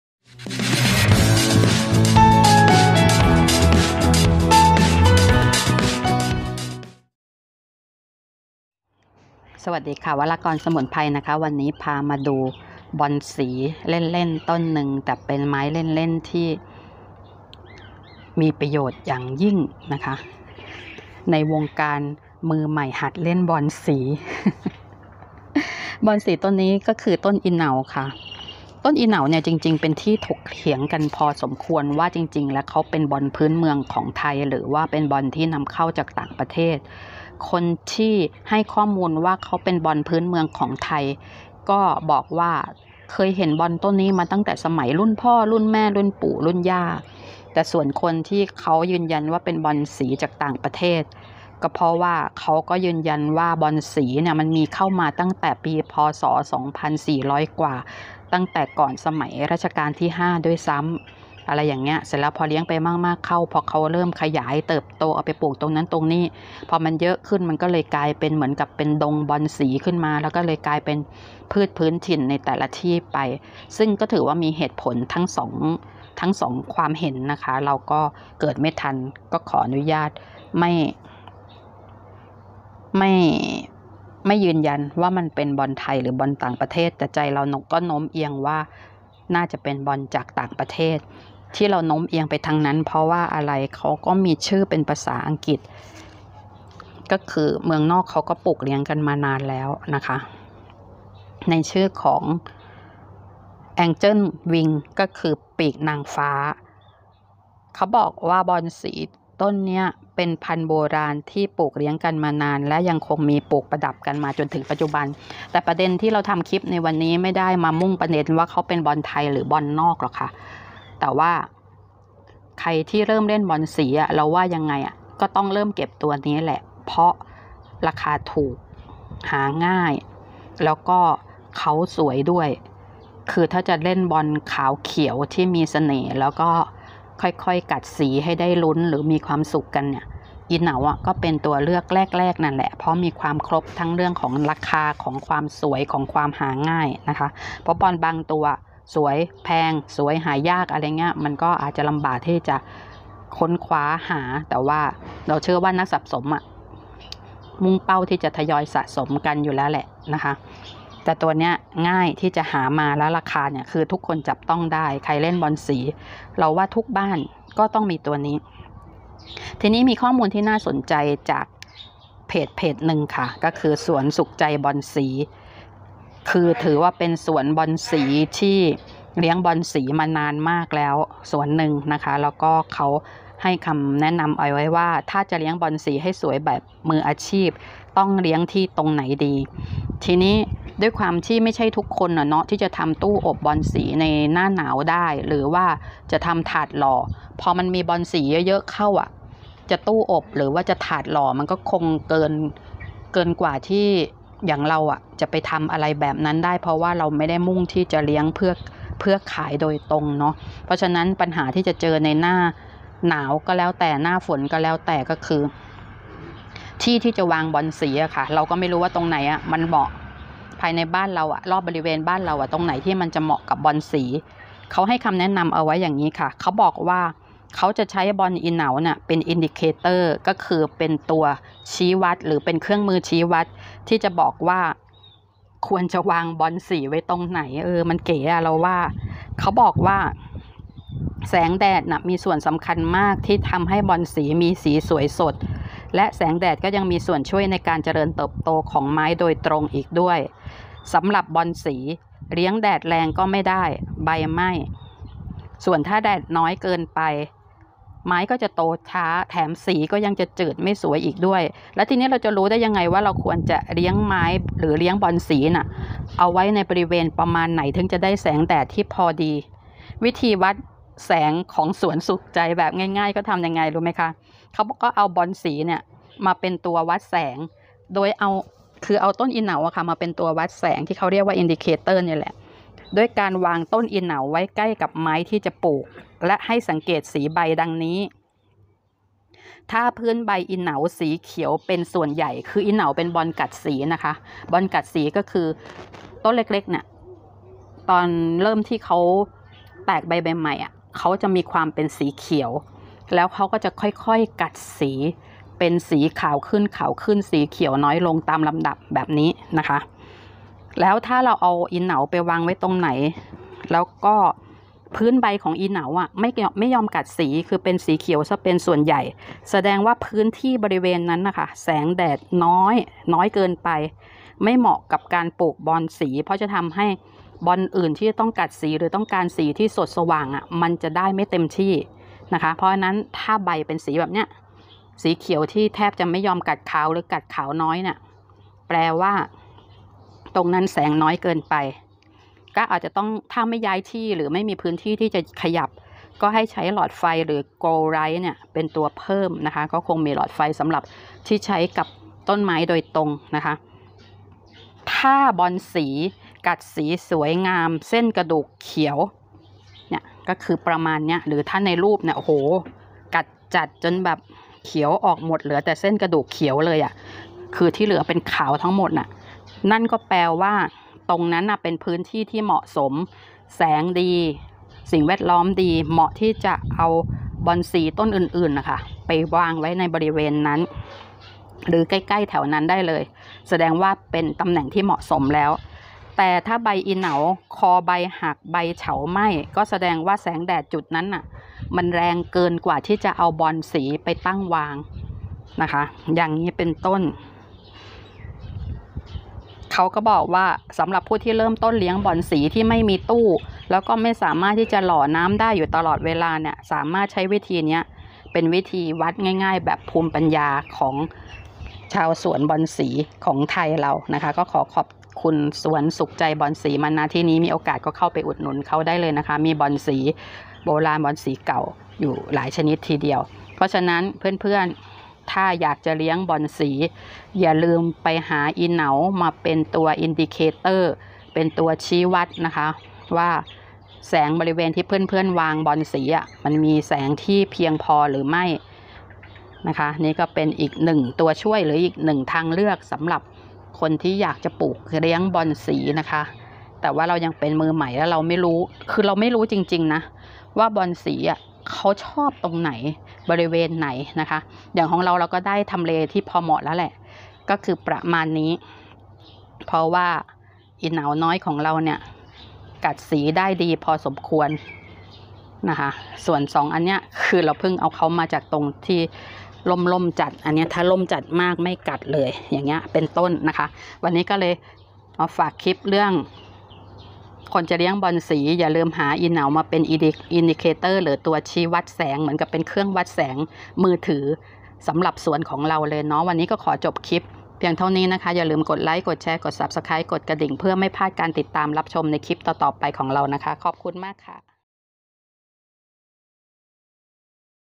บอนสีอิเหนา Caladium 'Candidum ตัวชี้วัดของบอนสี indicator of caladium with Thai wisdom
Caladium 'Candidum , indicator of caladium with Thai wisdom.

With green and white heart-shaped leaves, 'Candidum' Caladium is perfect for brightening up a shady spot. Its rich foliage thrives in containers and its moonlight-catching leaves can help define walkway borders after sunset. (Caladium 'Candidum')